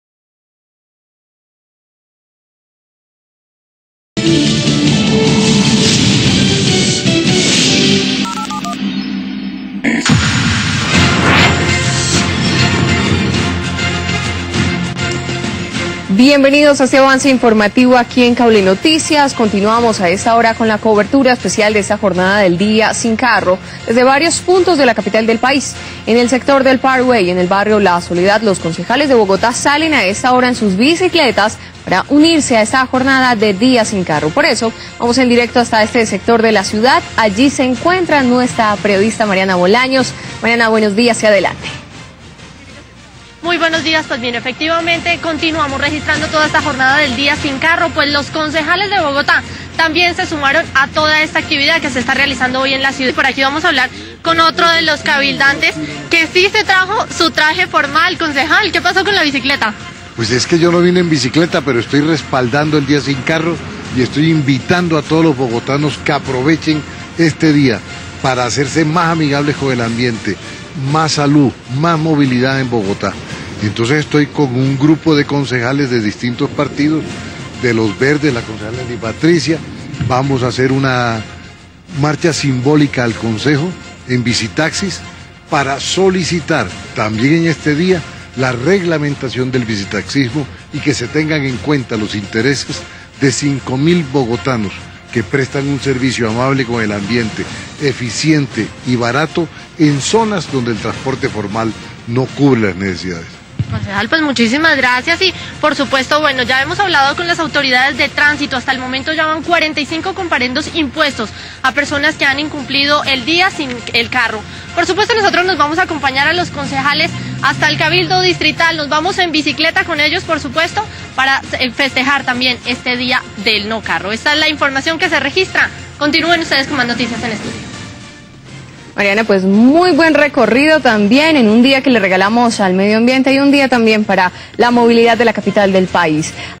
61. Día Sin Carro
El concejal progresista, Yezid García Abello, se movilizó hoy, Día Sin Carro, en bicitaxi y reclamó la reglamentación de este medio de transporte.